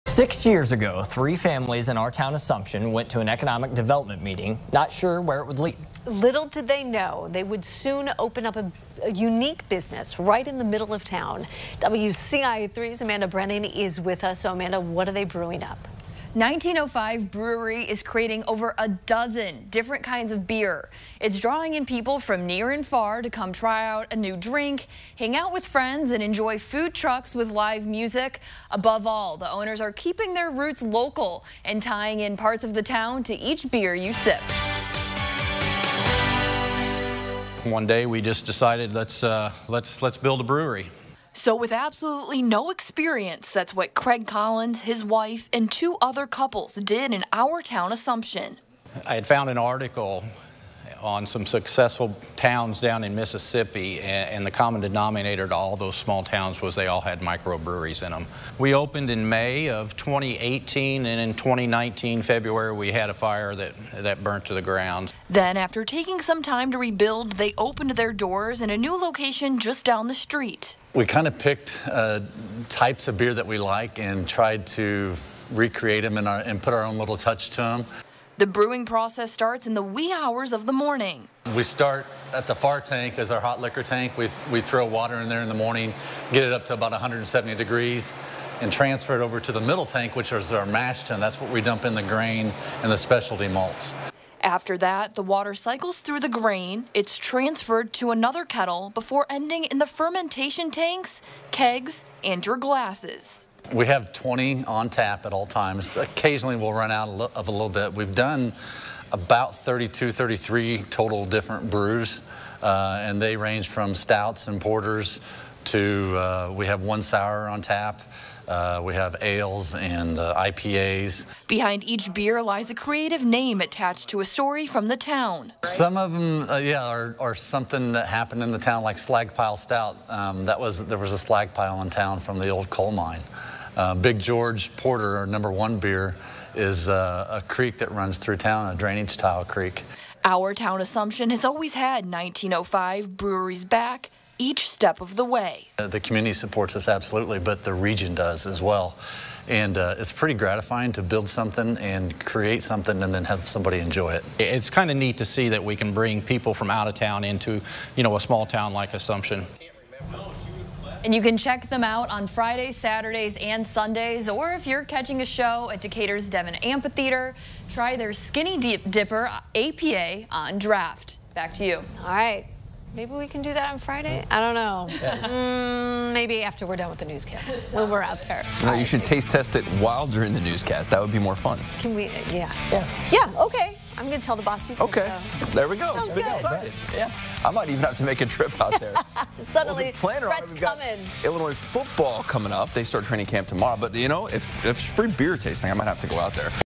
Something's brewing in Assumption, and each drink ties into the town
Six years ago, three families in Our Town Assumption went to an economic development meeting not sure where it would lead. Little did they know they'd soon open up a unique business right in the middle of town.